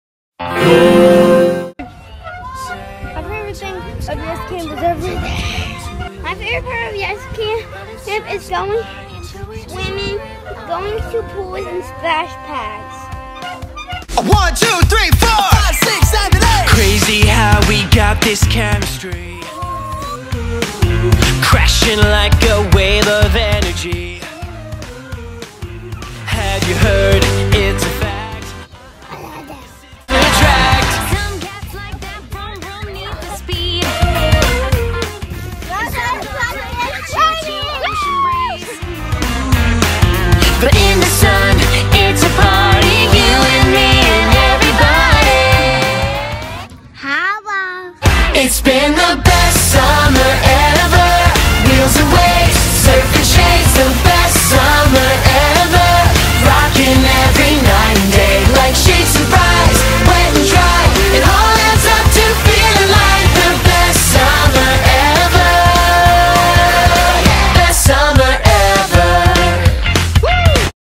The Arc's YESS Programs Astro Tots: "Best Summer Ever"
The Arc's YESS program is back in action this summer and the students, staff and volunteers are super excited and will tell you they are having the "Best Summer Ever"

Each group made a quick video highlighting some of their fun this summer.